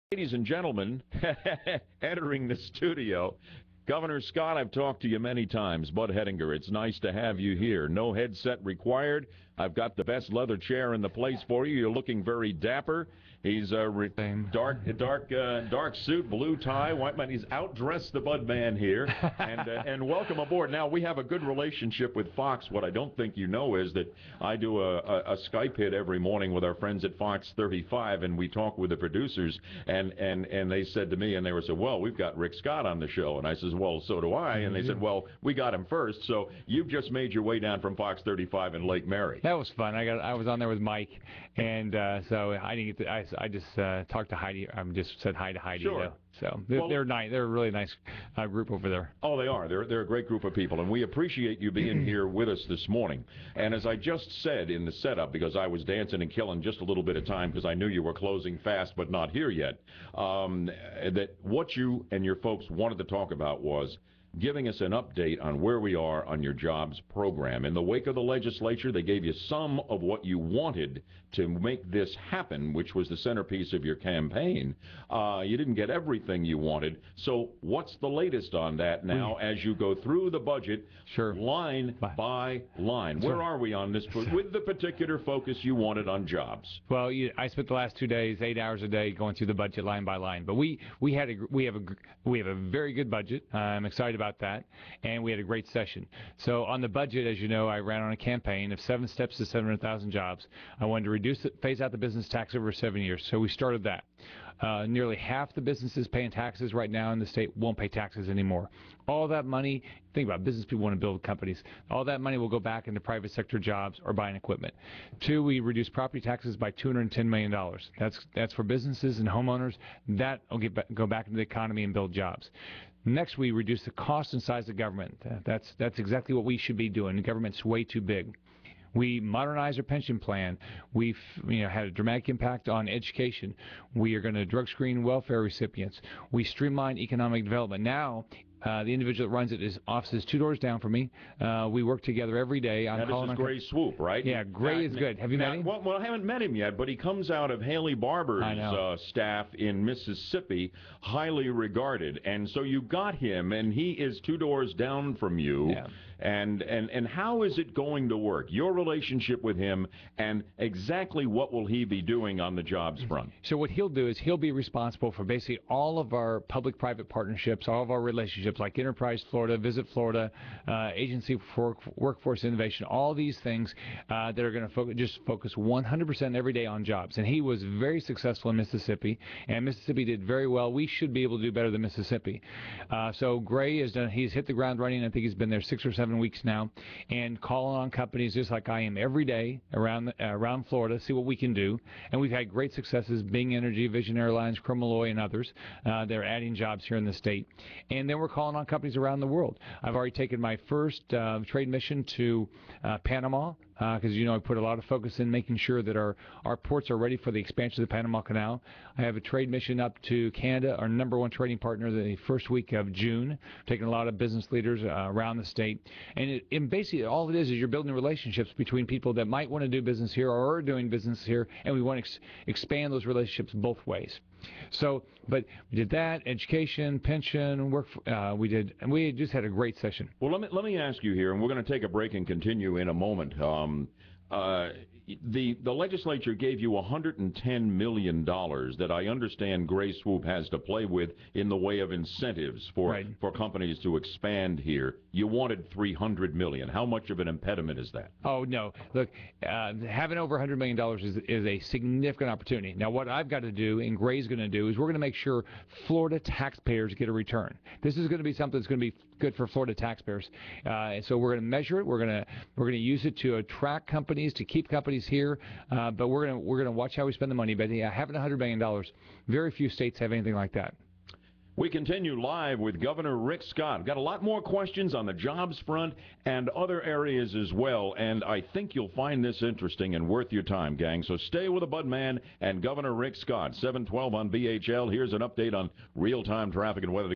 BHL Rick Scott Interview Part 1
Governor Rick Scott joins the Budman! -- 540 WFLA Orlando